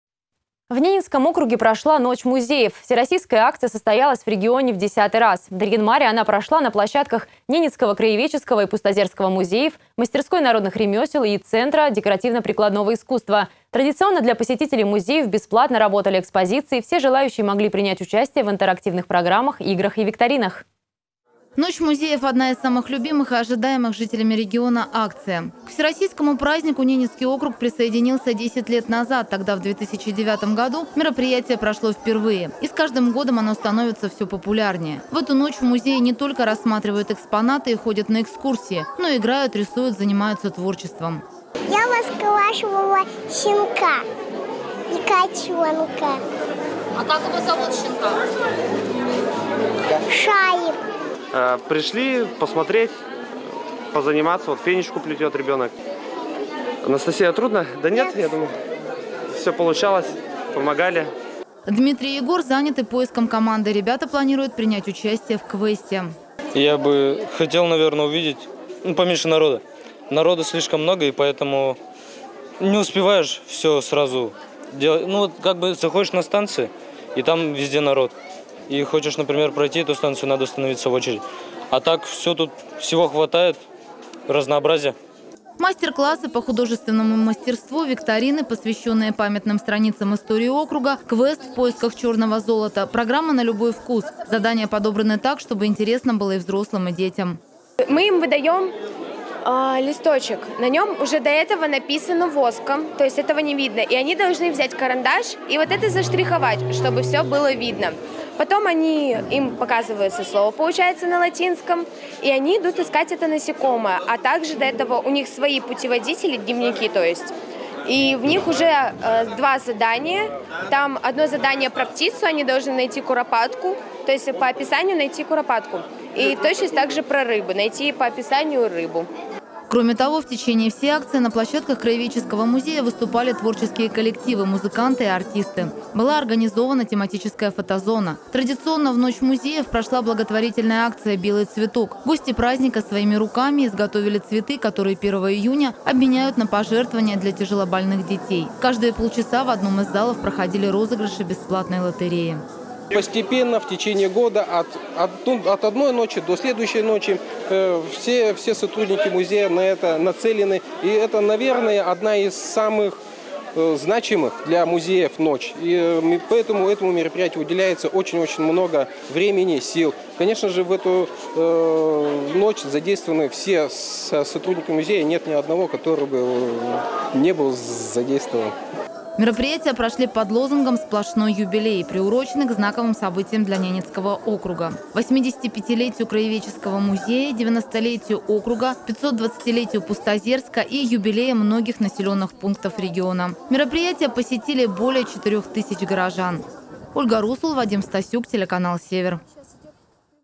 В Ненецком округе прошла акция «Ночь музеев»
Всероссийская акция «Ночь музеев» состоялась в регионе в десятый раз. В Нарьян-Маре она прошла на площадках Ненецкого краеведческого и Пустозерского музеев, Мастерской народных ремёсел и Центре декоративно-прикладного искусства. Традиционно для посетителей музеев бесплатно работали экспозиции, все желающие могли принять участие в интерактивных программах, играх и викторинах.